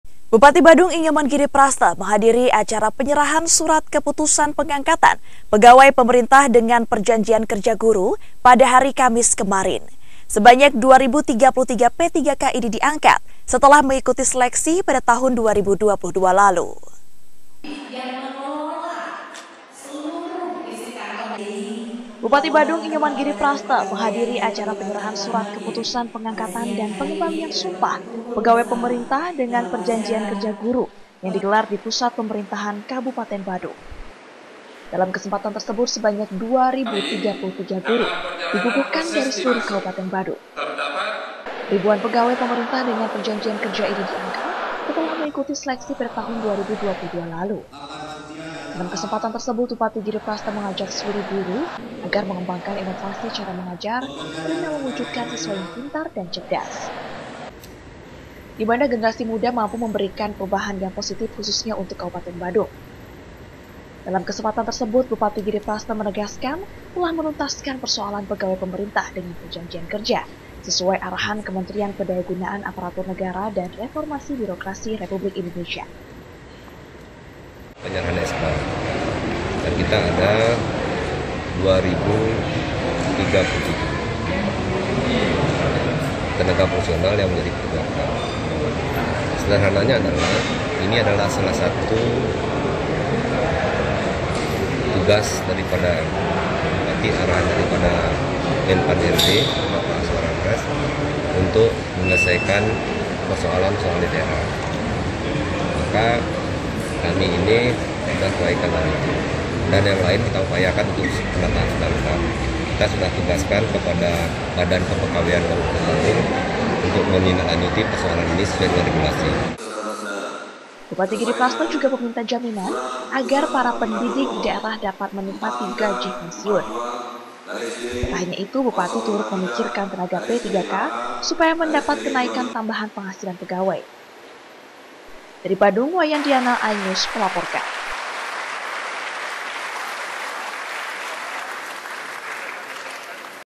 Bupati Giri Prasta Ajak Guru Kembangkan Inovasi Dalam Cara Mengajar
Badung - Lintas iNews Bali, 8 September 2023, Bupati Giri Prasta Ajak Guru Kembangkan Inovasi Dalam Cara Mengajar